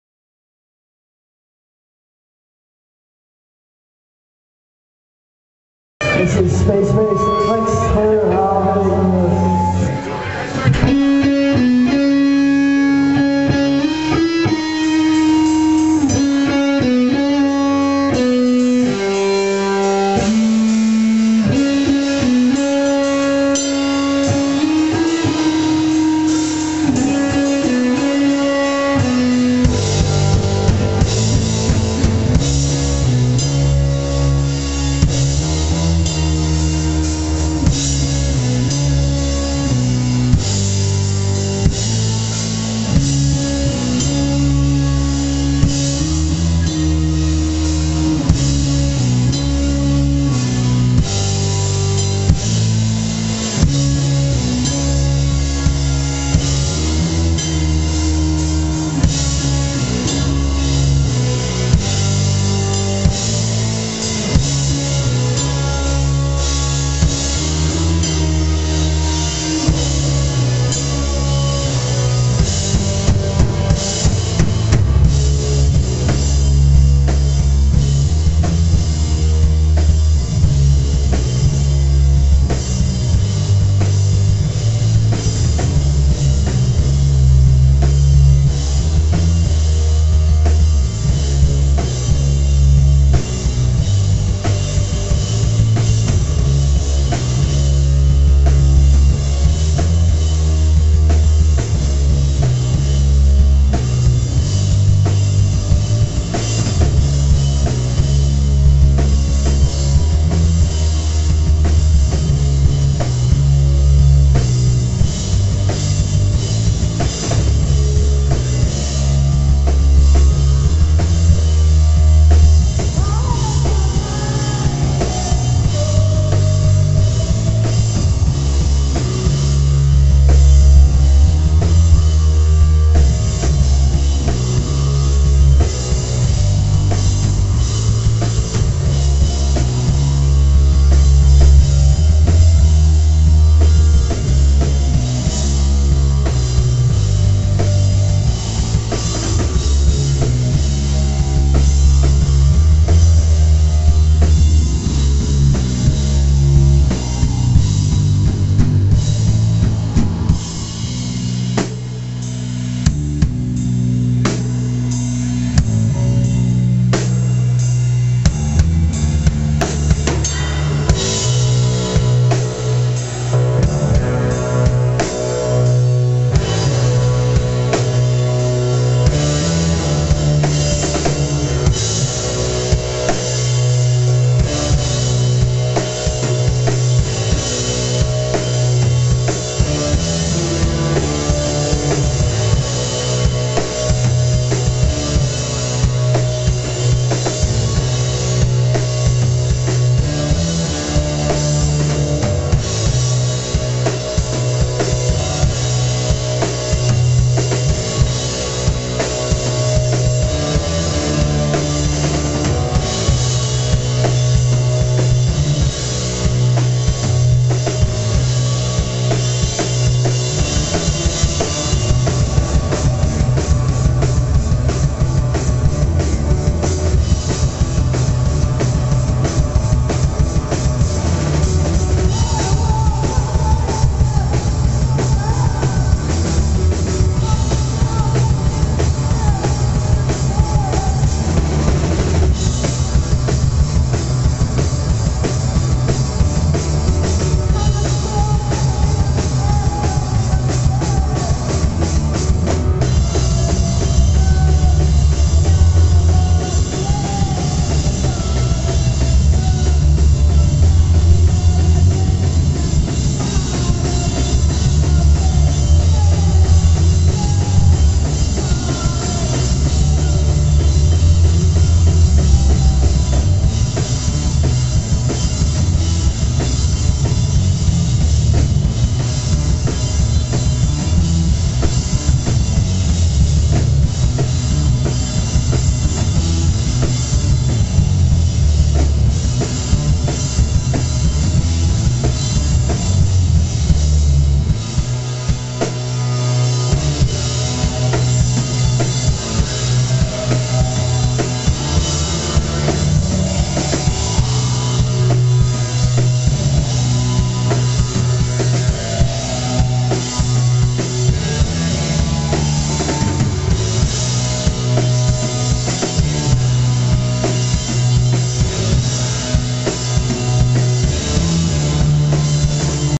Wild Rocket at Richardsons,Galway
Wild Rocket , newstarts from Dublin launch their sonic attack on the west in Richadrsons,Eyre Square,Galway on Saturday,Jan 14th 2012